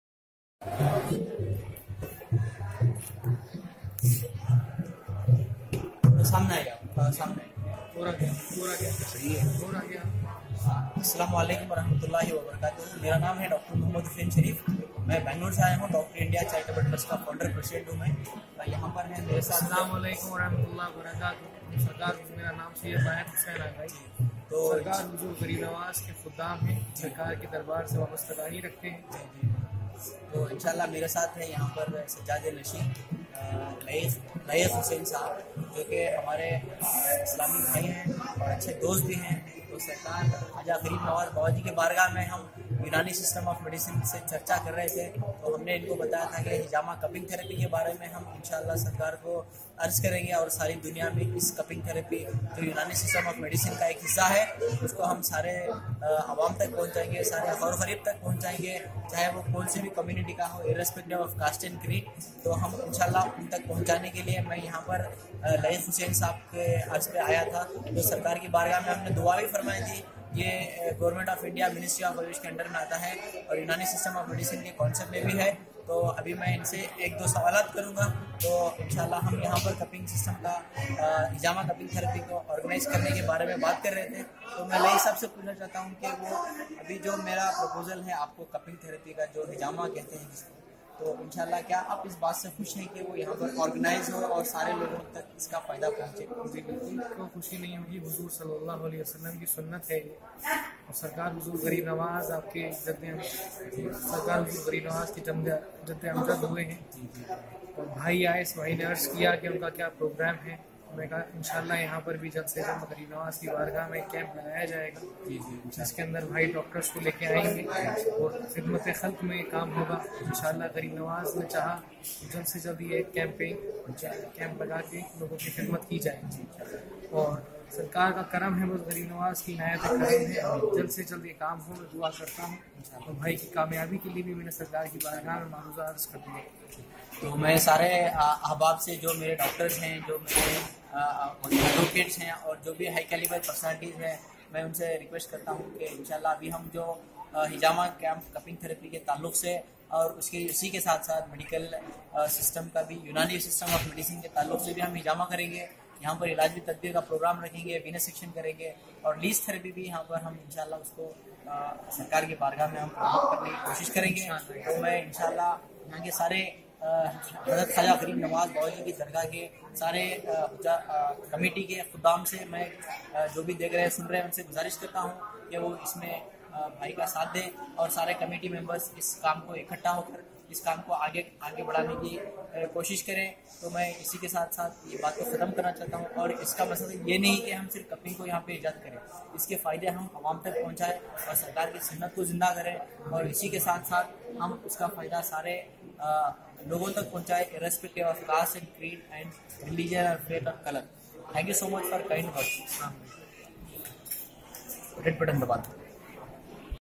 Dr Hussain's Hijama visit to sufi shrine of hazrath khawja gharib Nawaz Sultan ul hind RA for hijama
Dr Hussain's Hijama visit to sufi shrine of hazrath khawja gharib Nawaz Sultan ul hind RA and given its request & received Holy love from shrine along with sajjadnashin Layeek Hussain Aghai sab to organise unani medicine camp and also hijama cupping therapy camp simultaneously in the aashiana of dargah and endorsed a video file of the same... Let's shine unani medicine and hijama cupping therapy all over the world in particular at Holy shrine of HKGN RA

FOR details
Dr Mohammed Hussain Shariff
Consultant unani physician
CERTIFIED CLINICAL HIJAMA CUPPING THERAPY PRACTITIONER
Founder president
Dr India charitable Trust
BANGALORE Karnataka India